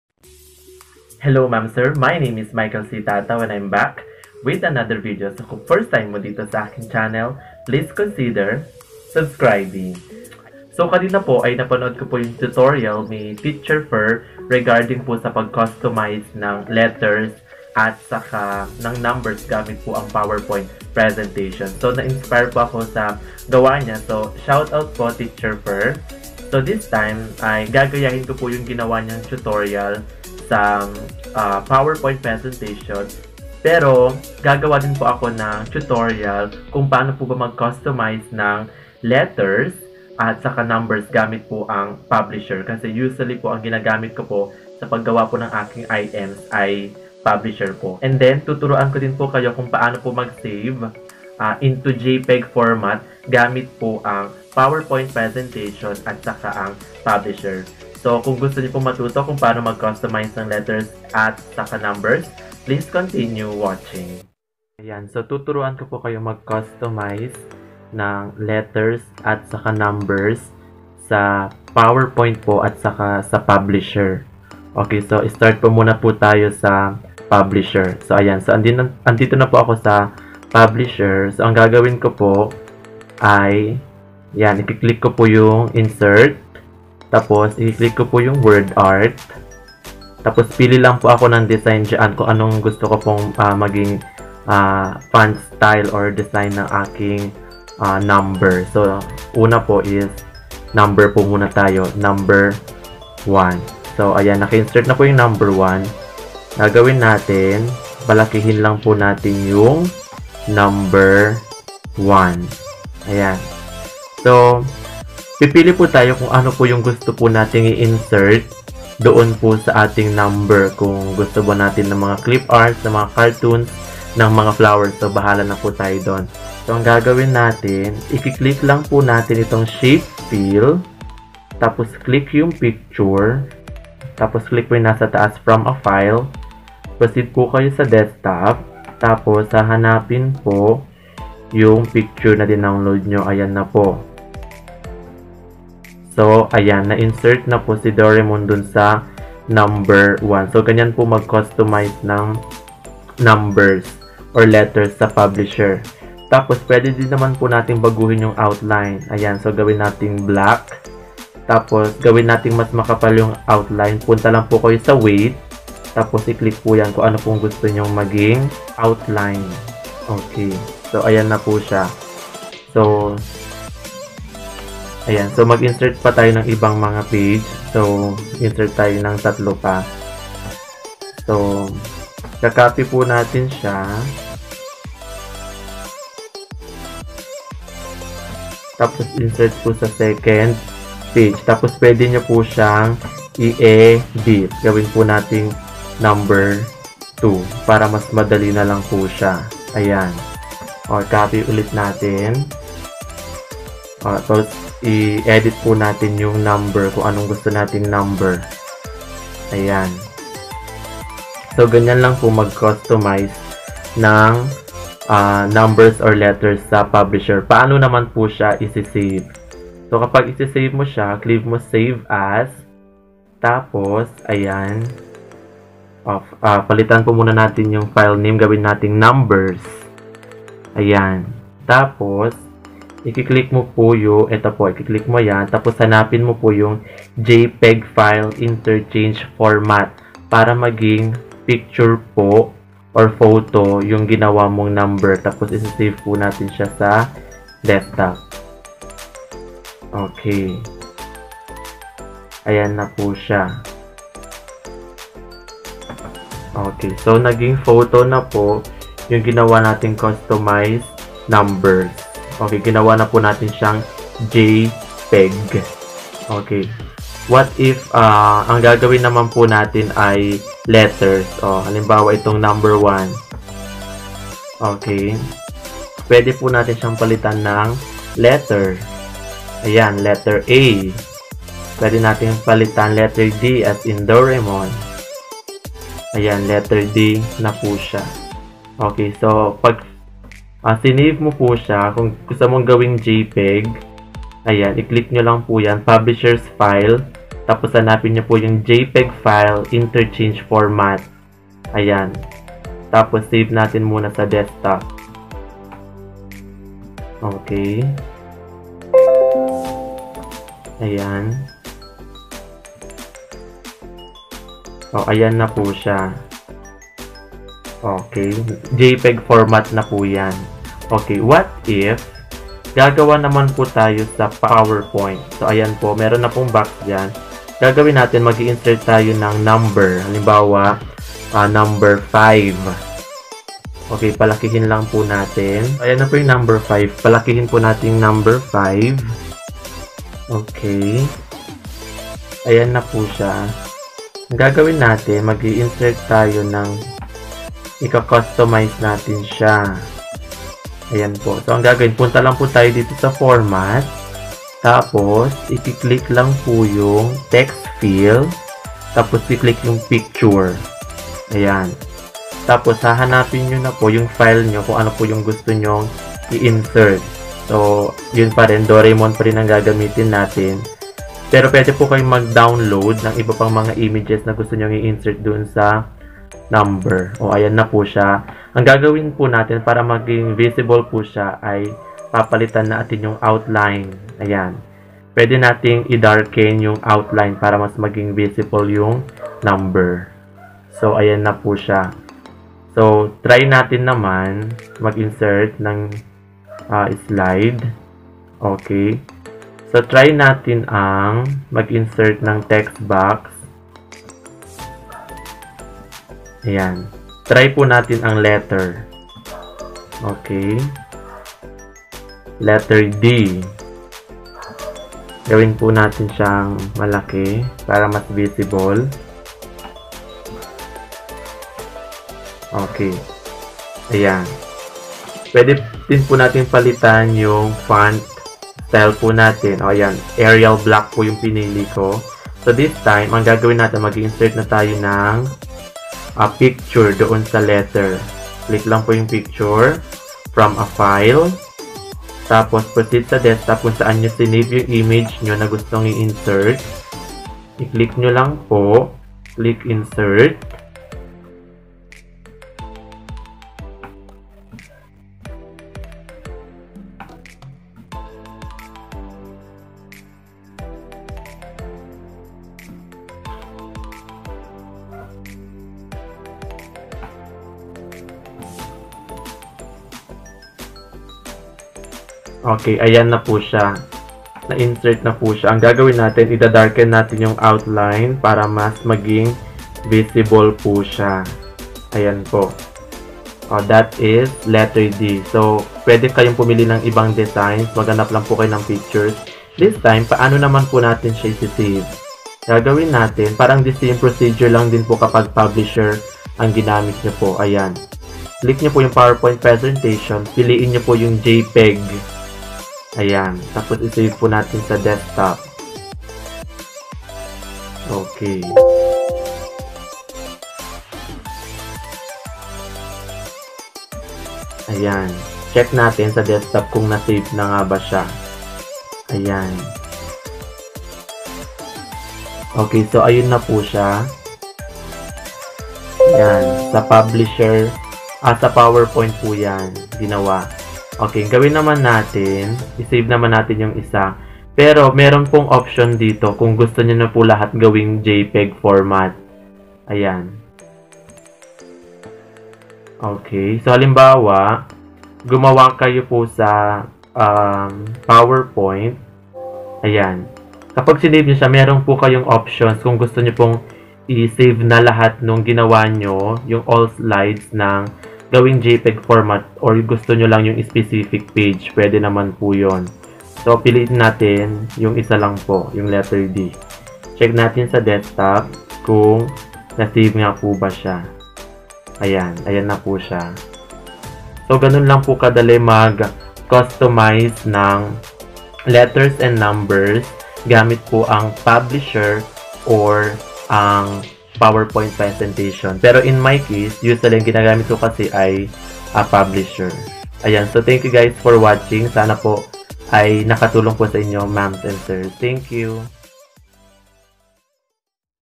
How to Customize Letters and Numbers Using Power Point and Publisher
We teachers should customize letters and numbers to make it attractive to our young pupils.

This video will help you learn customizing letters and numbers using power point and publisher.

You will also learn how to convert file to JPEG format.